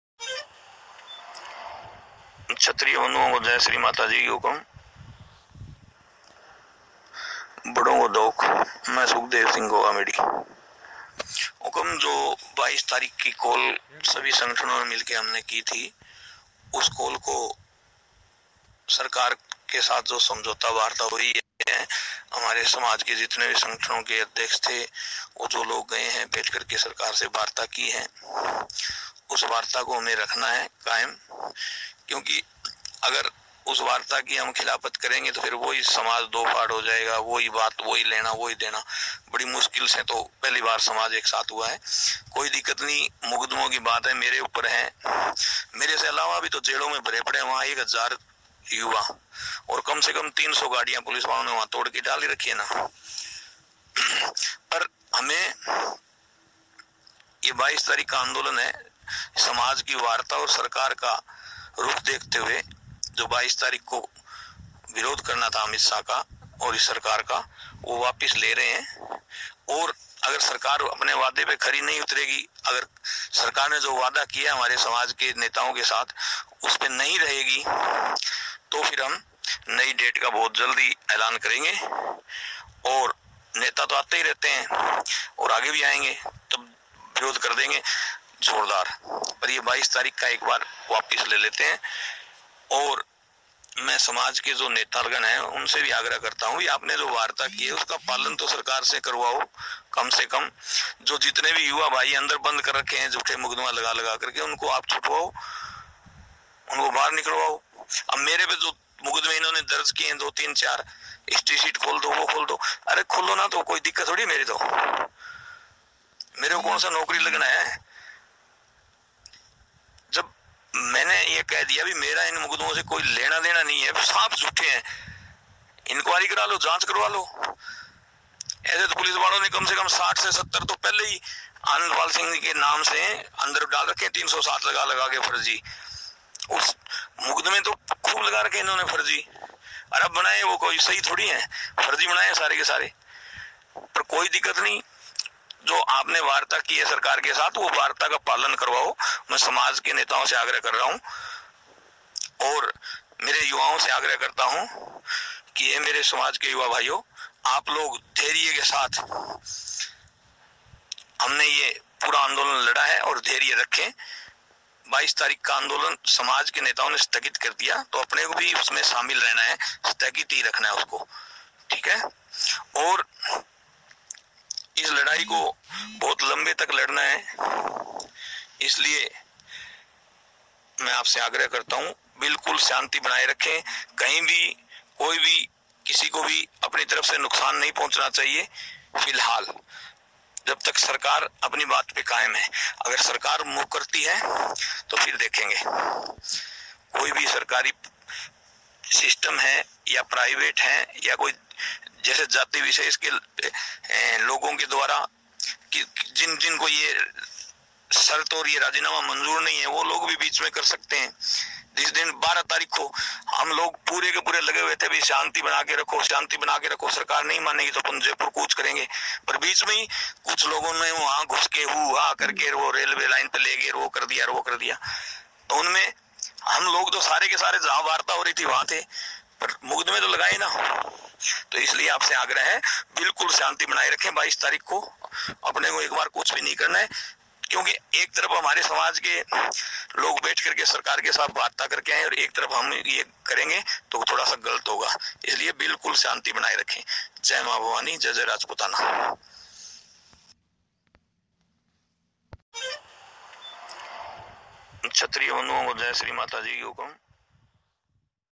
Rajputs jaipur railly:आखिर खत्म हुआ इंतेजार सुखदेव सिंह गोगामेड़ी का आया ऑडियो दिया युवाओ को संदेश
आखिर खत्म हुआ इंतेजार सुखदेव सिंह गोगामेड़ी का आया ऑडियो दिया युवाओ को संदेश,anandpal encounter,amit shah in jaipur,sukhdev singh gogamedi,rajput karni sena,rajasthan latest news